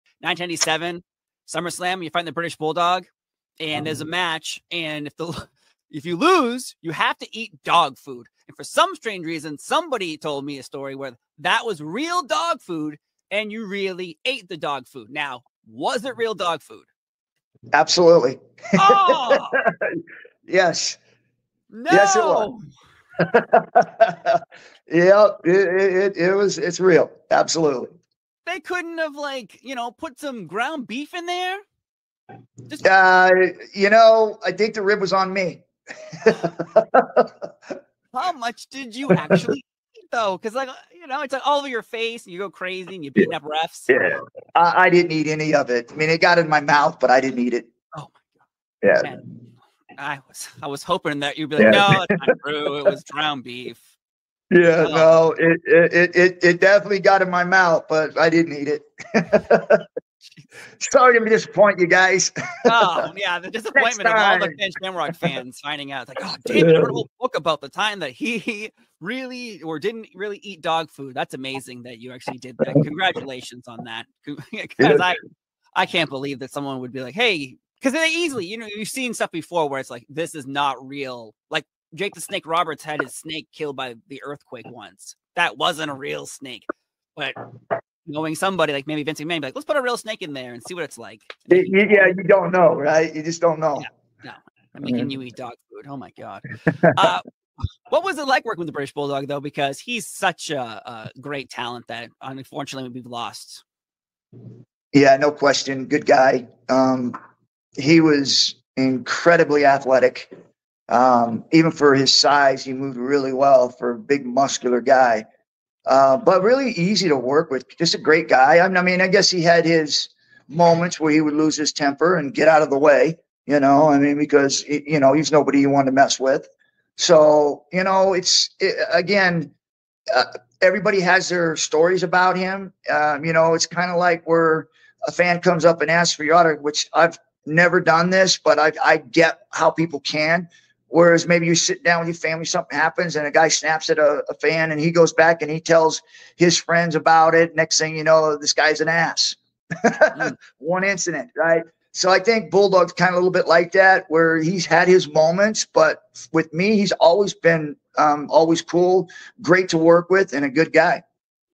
Ken Shamrock SHOOTS on How He Feels About WWE’s British Bulldog
Ken Shamrock reveals the truth about eating real dog food during his match against the British Bulldog at WWE SummerSlam 1997. He discusses the experience, how much he actually ate, and the rib played on him. Shamrock also shares his thoughts on working with the British Bulldog, praising his athleticism and character. Don't miss this candid conversation with the most dangerous man in the world!